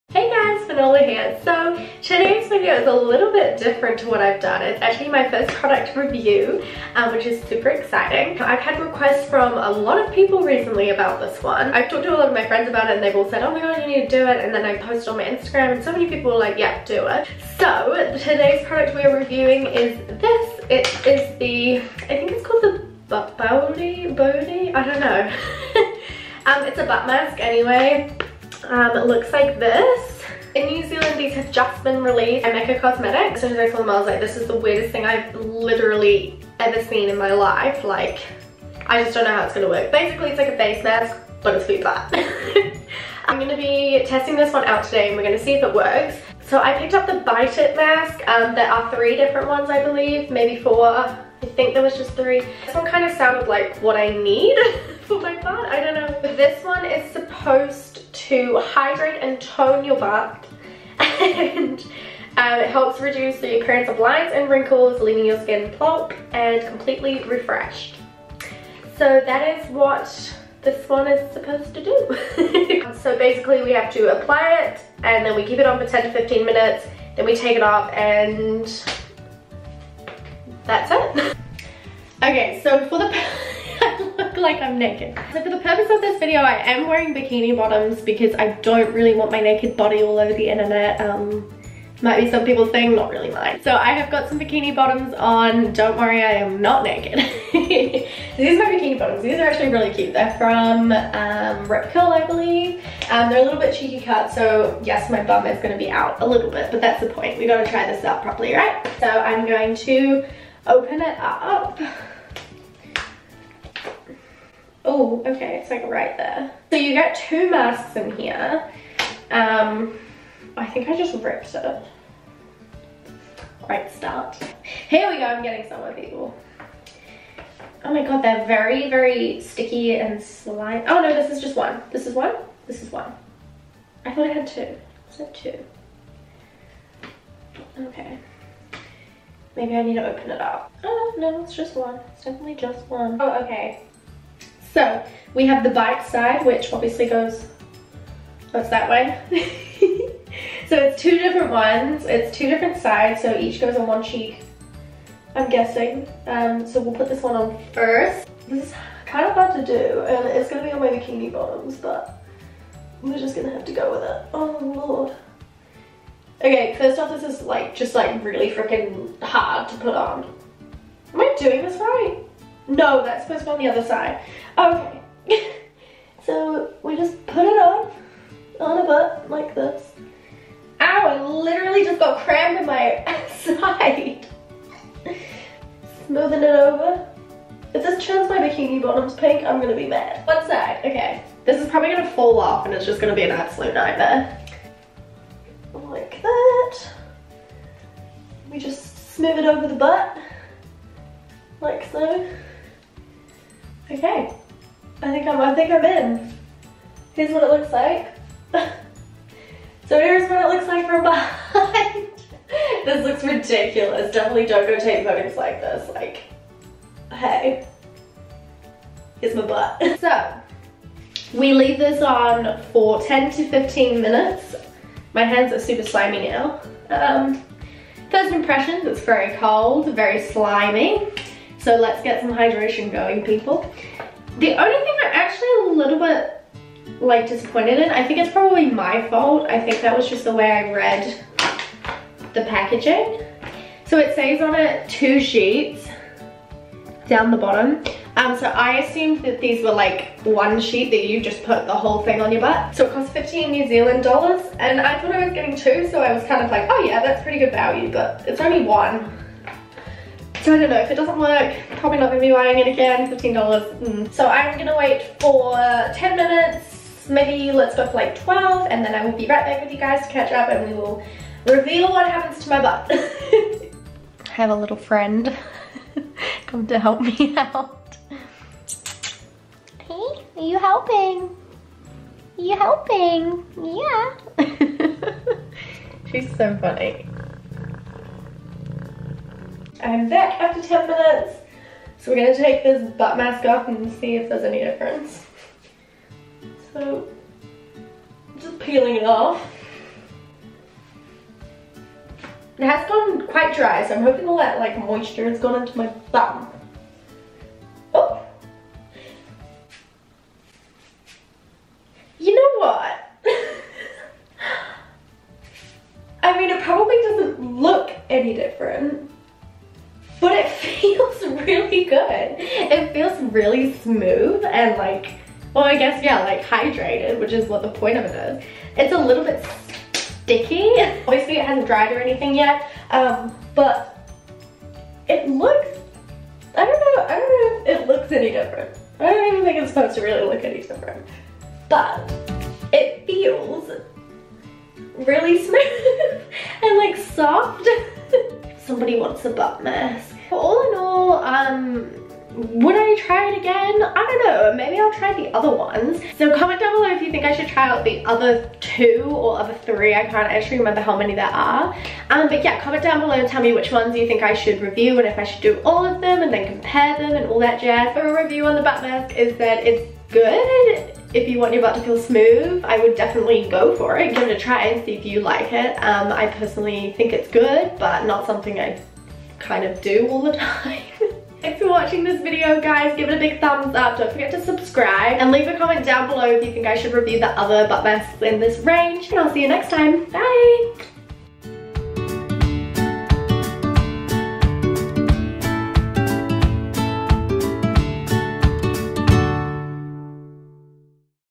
Bawdy Butt Mask Review - WTF?
Hey guys!

Check out what happened when I reviewed the Bawdy Bite It Butt Mask!

A mask... for your butt??

Shall I review another one?

If you are a company interested in working with me, or would love me review your product, feel free to contact me via my Instagram @fynnola (Currently working on an email account for promotion work :) )